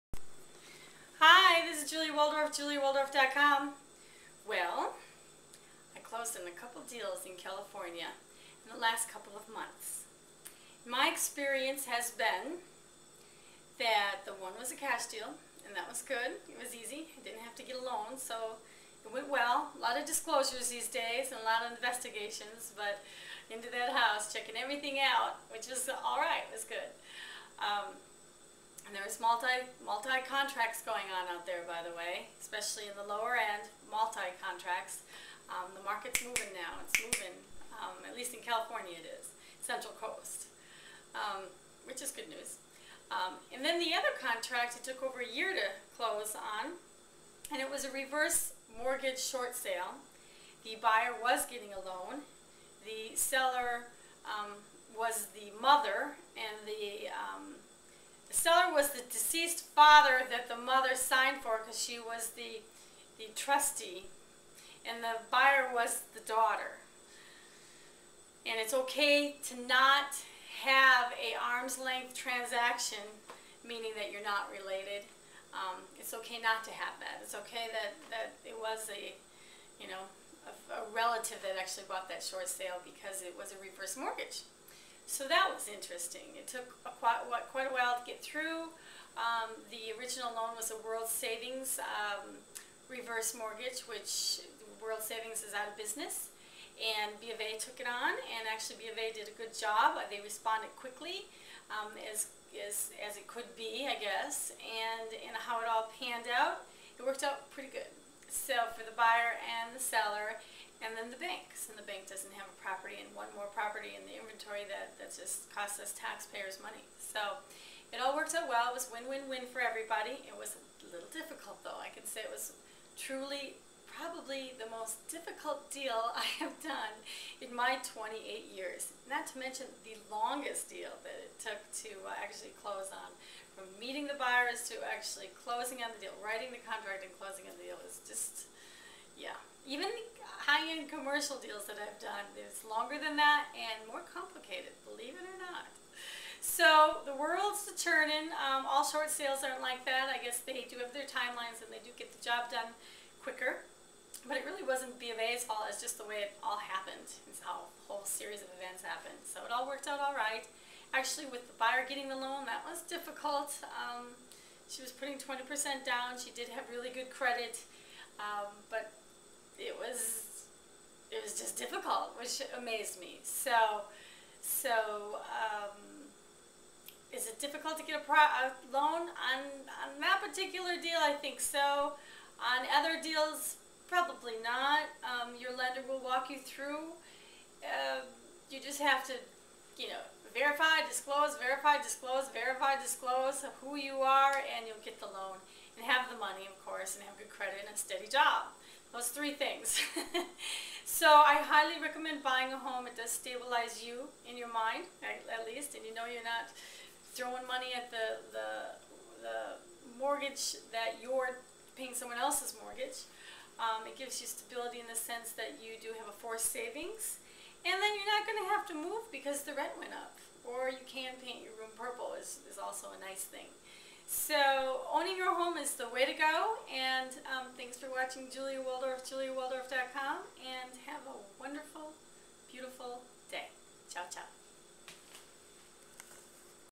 Closing the Deal!
talks about the two real estate deals she has closed on in Central Coast California.  See more Central coast California real estate, fun in the sun, single parents, homeless in San Luis Obispo